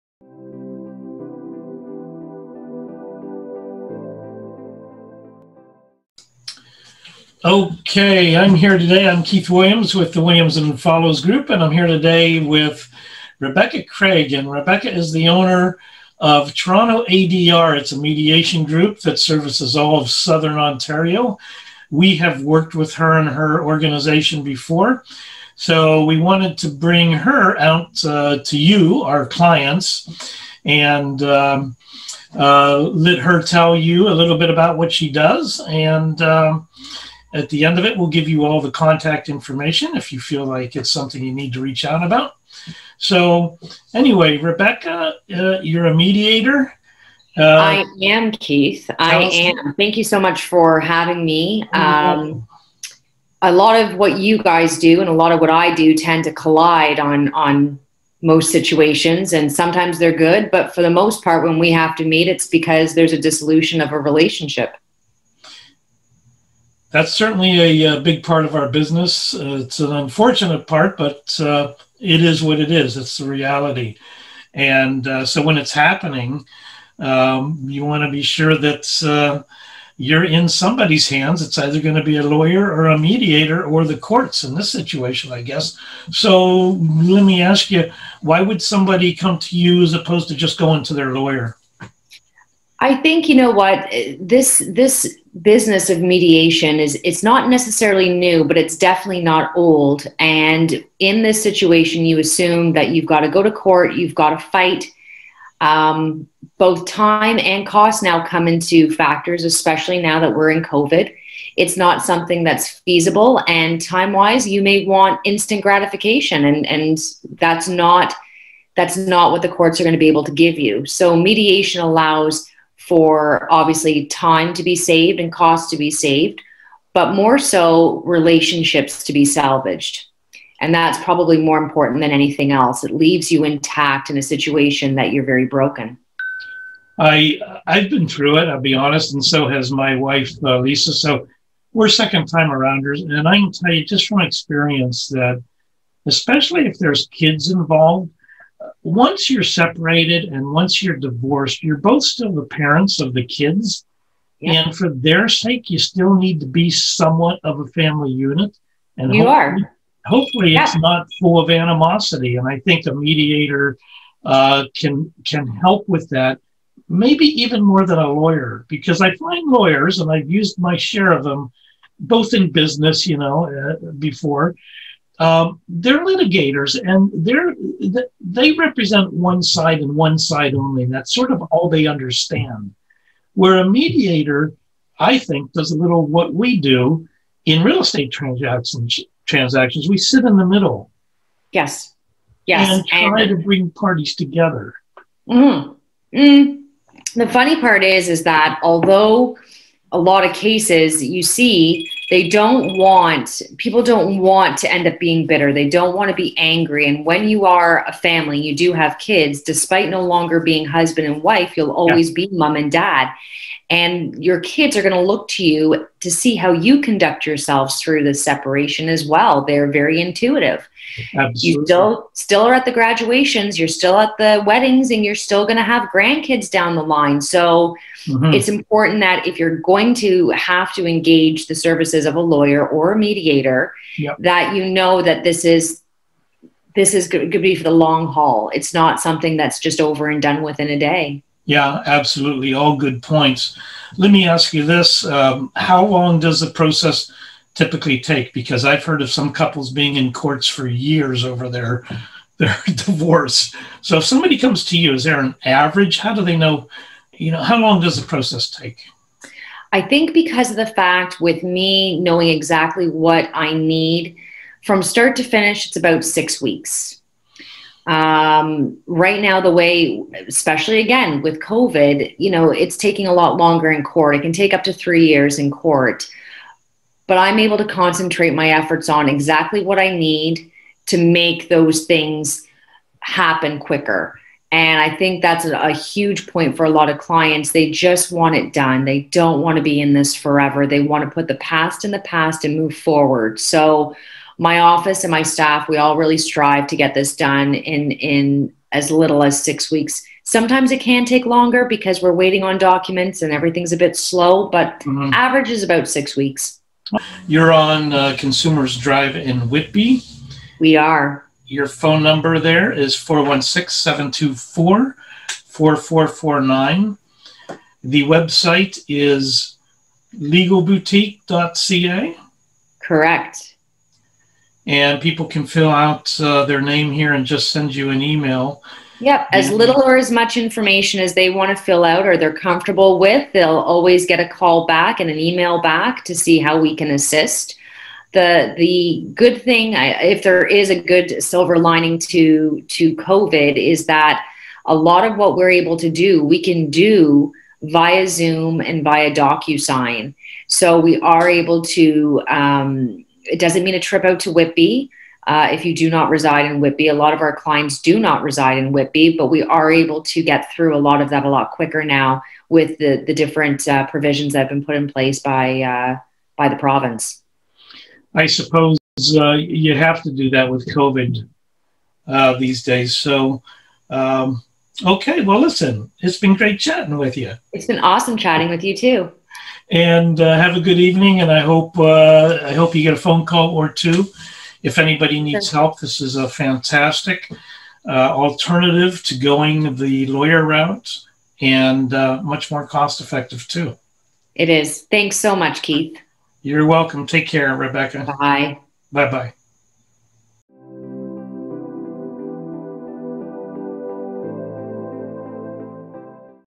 Keith Williams chats with our trusted service partner Rebecca Craig from Toronto ADR Services
Connecting with our community and extended team is important to us! Join our own Keith Williams as he chats with our trusted service partner Rebecca Craig from Toronto ADR Services, a highly regarded mediation group located in Durham Region. Rebecca and her team provide essential support to our team at Williams & Follows and to many of our clients who are experiencing divorces or separations while selling their homes.  They are currently assisting their clients with online services for family mediation, civil & mandatory mediation, document commissioning, last will and testaments, divorces, separations and more.

We can’t recommend them enough for their professionalism and trusted service.